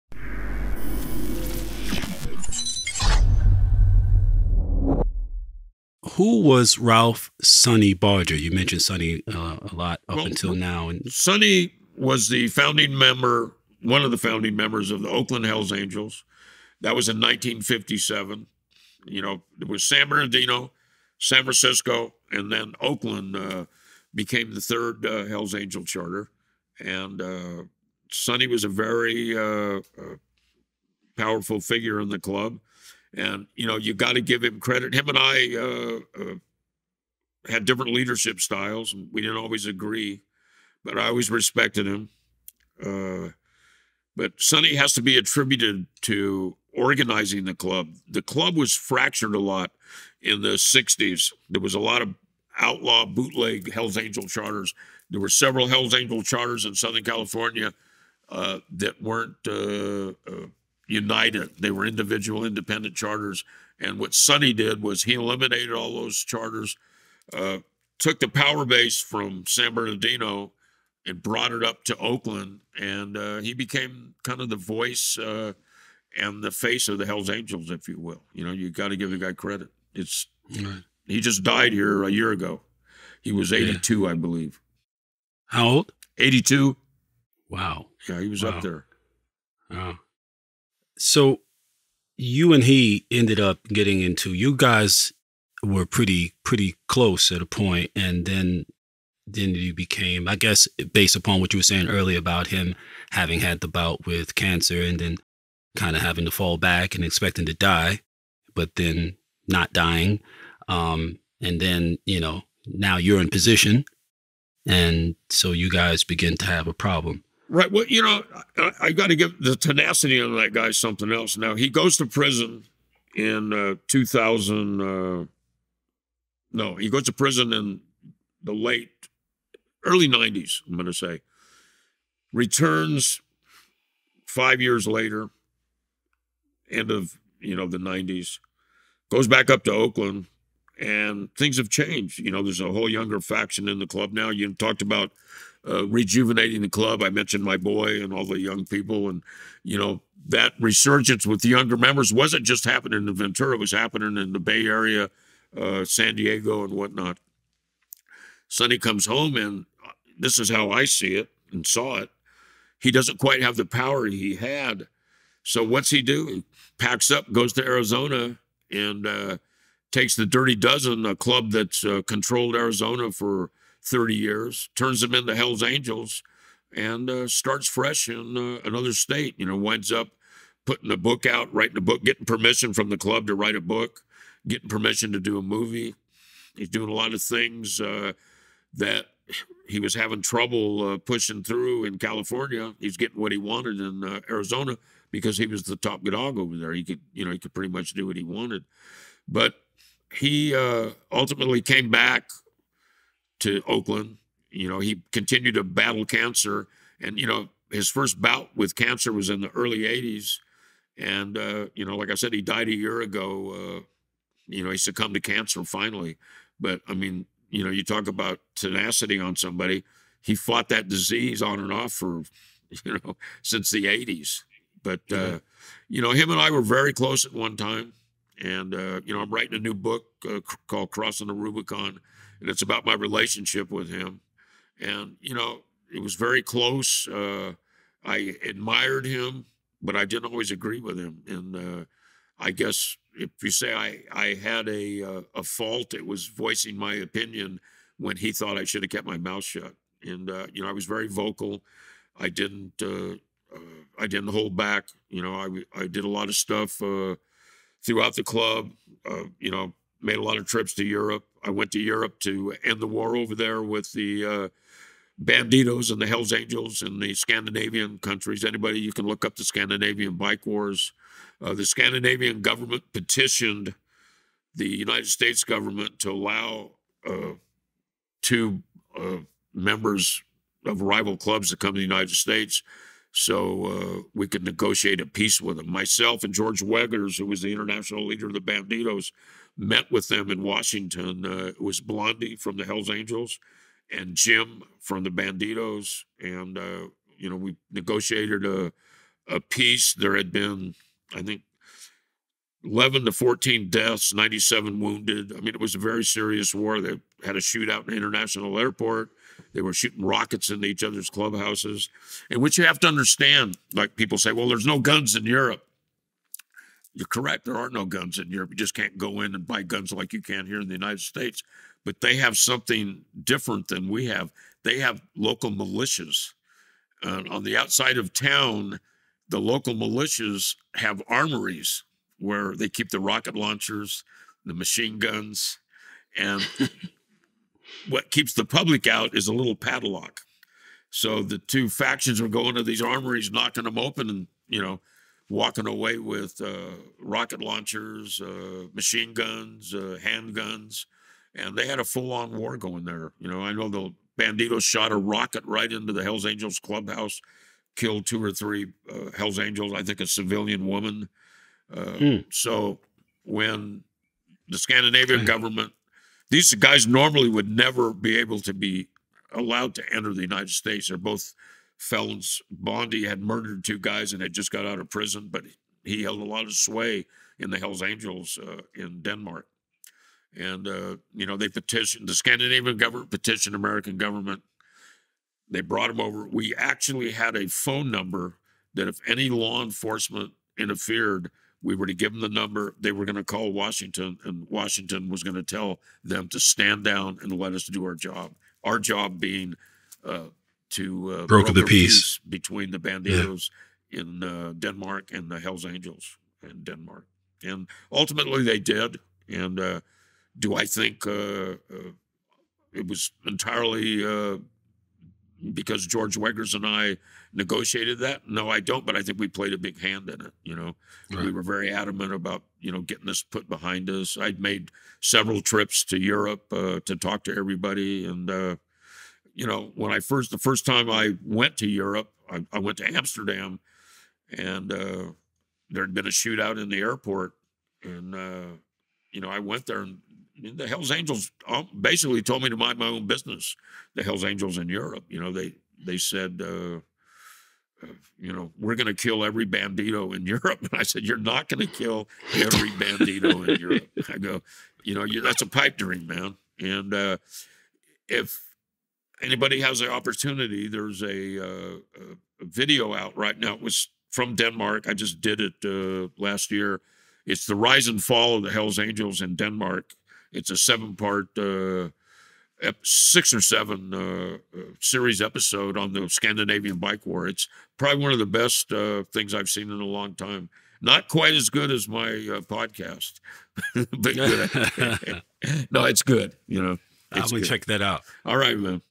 George Christie on the Nordic Biker War w/ Hells Angels & Bandidos, 14 Deaths & 97 Wounded (Part 12)
In this clip, George Christie detailed his relationship and eventual fallout with founding Hells Angels member Sonny Barger. Christie praised Barger's leadership and pointed out the late member's death after a lengthy battle with cancer. After revealing he was writing a book about his relationship with Barger, Christie reflected on negotiating peace in the years-long Nordic Biker War between the Hells Angels and Bandidos.

Interview by: Cavario